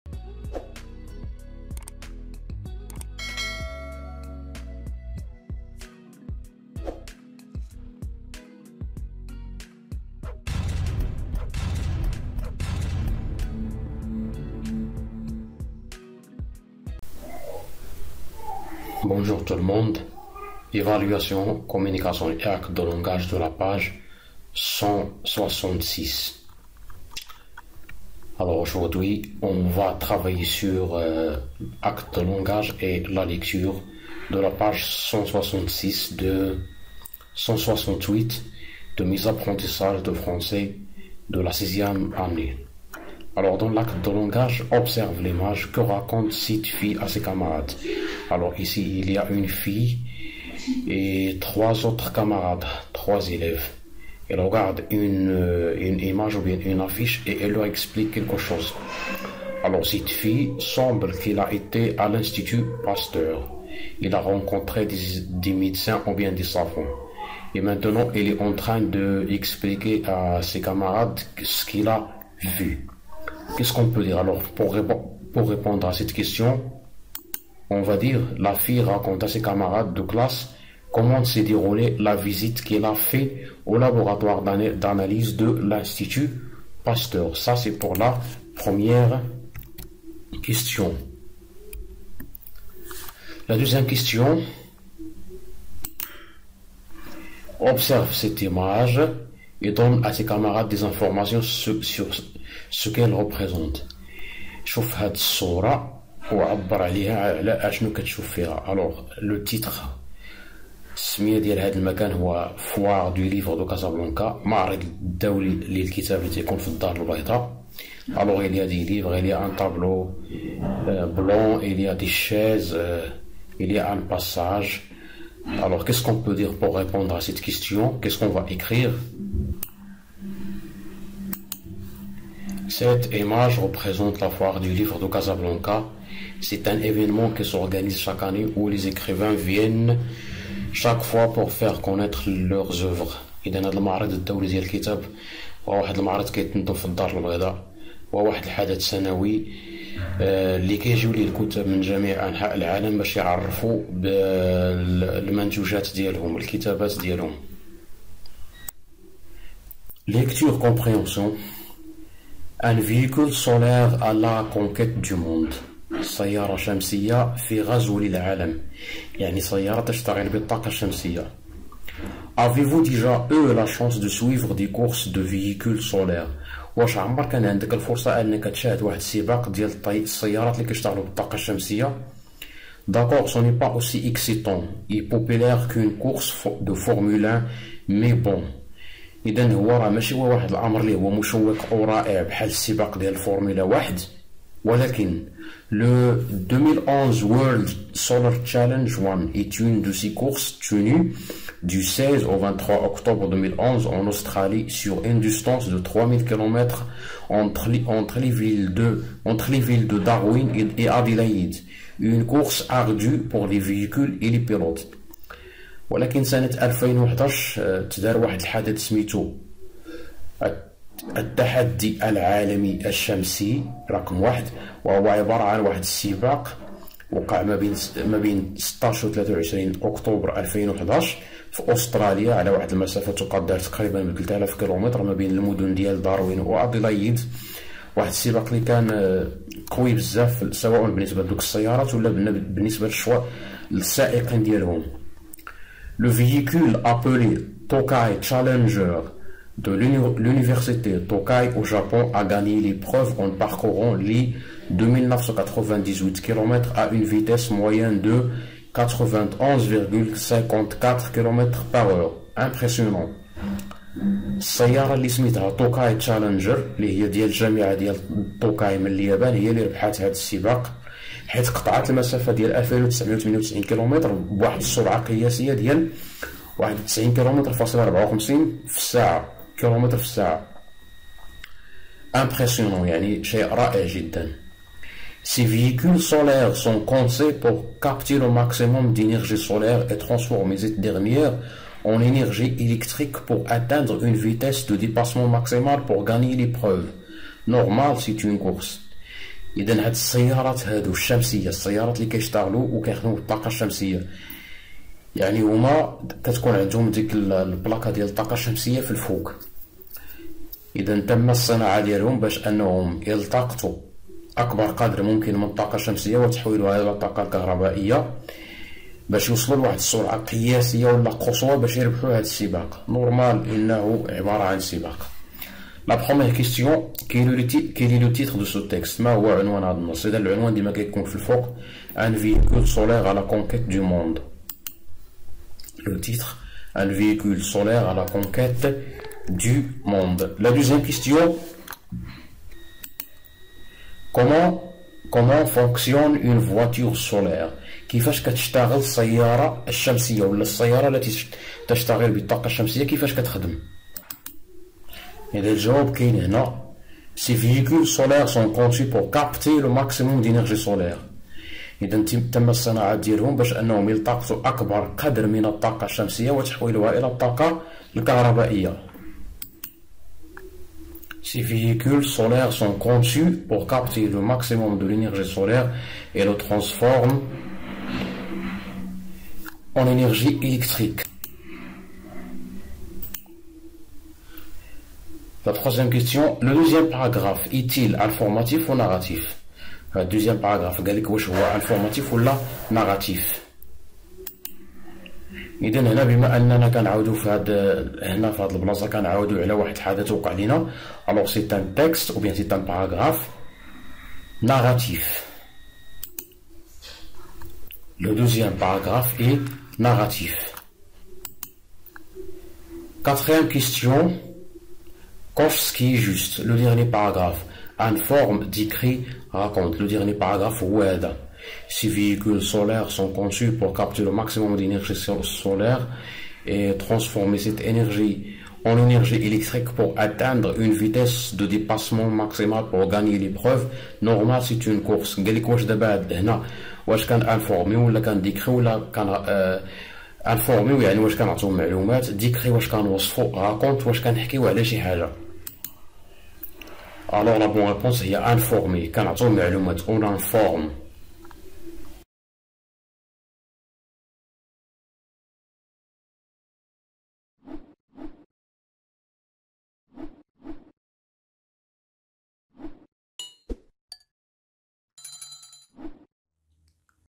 Mes apprentissages en français 6aep -PAGE 168 /évaluation actes de langage lecture
évaluation actes de langage + lecture : véhicule solaire à la conquête du monde
Mes apprentissages en français 6aep -PAGE 168